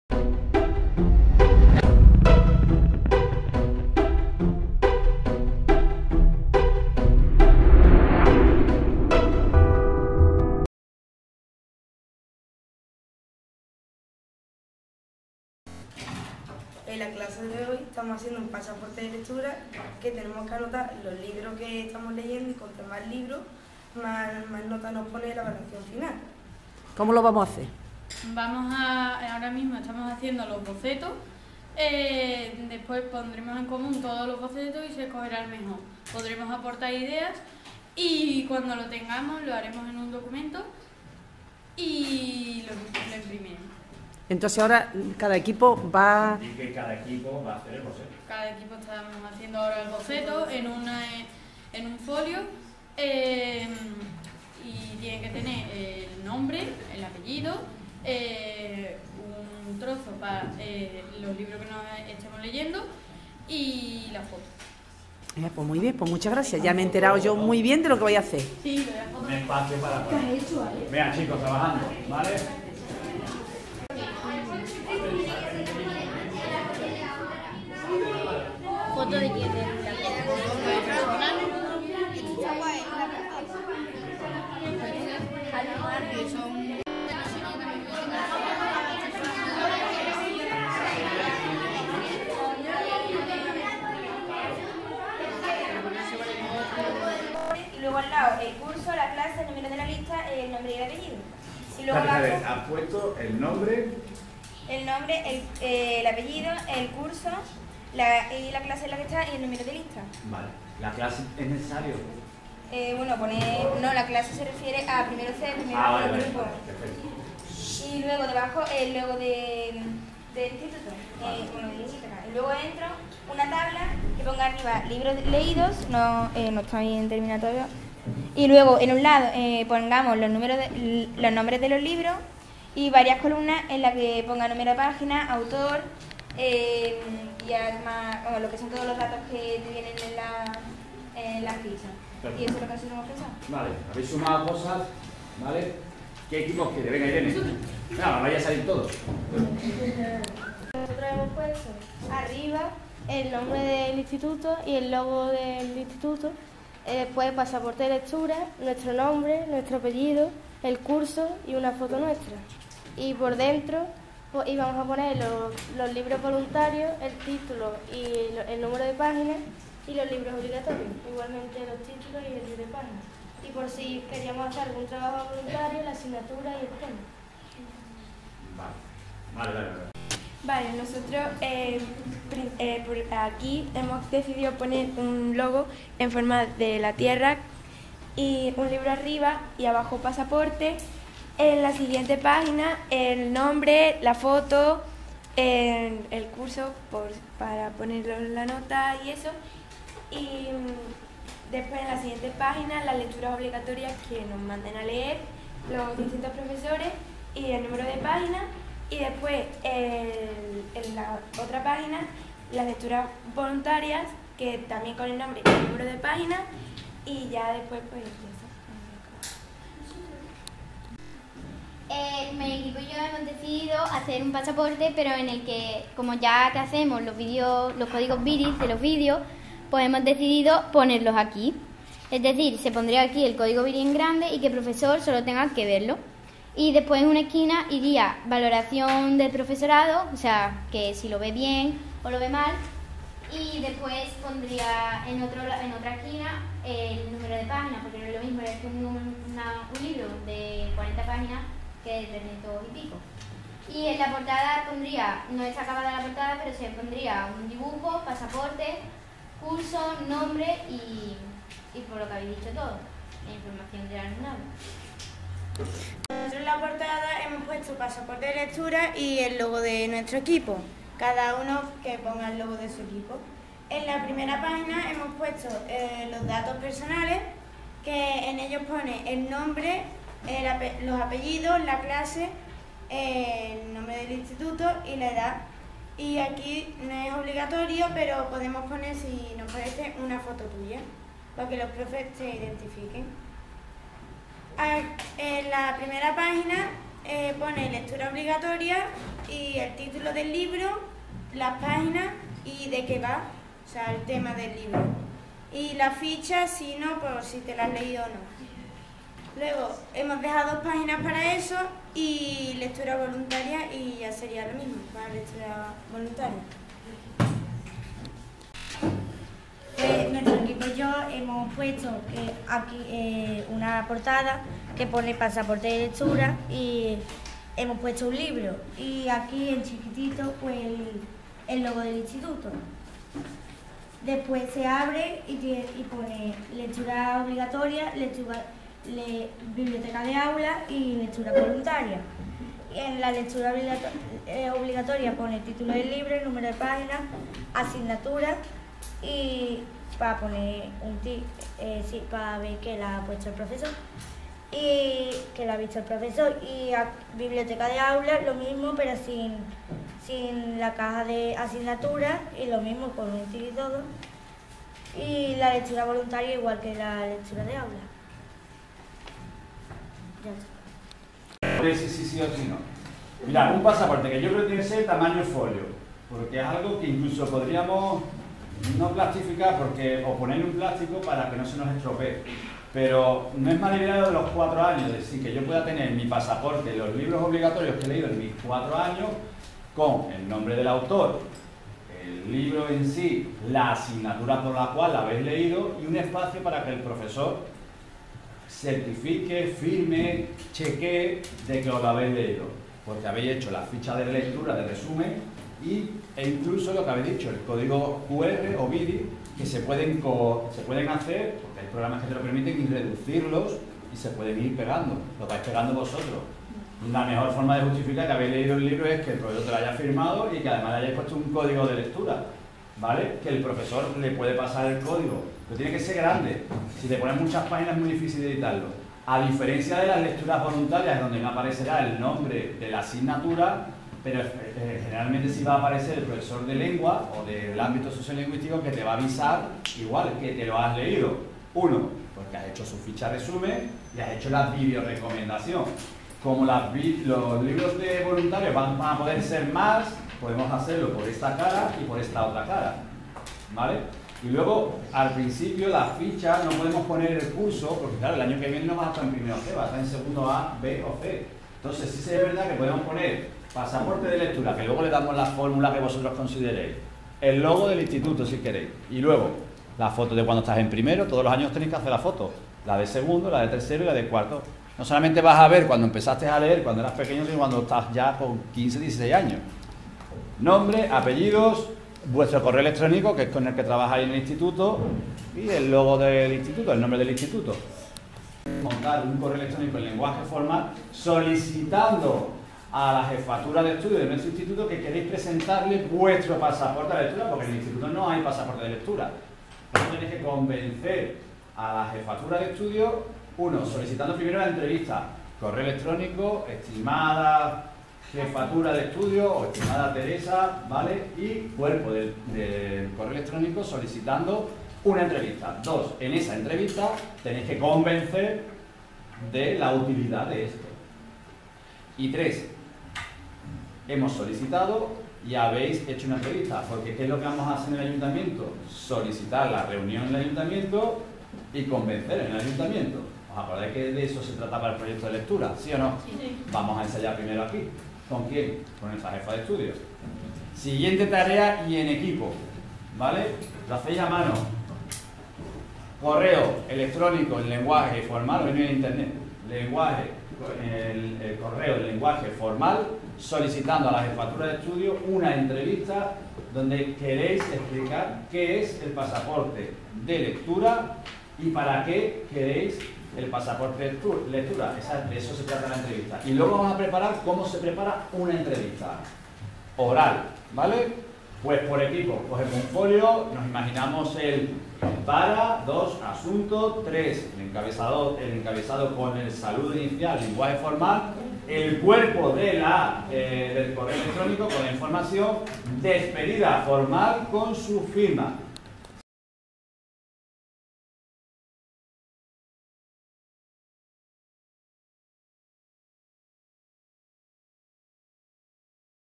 PASAPORTE DE LECTURA.
El alumnado de 1º de ESo, en la materia de ASL, con el profesor Juan Antonio Bascón, realiza el pasaporte de lectura.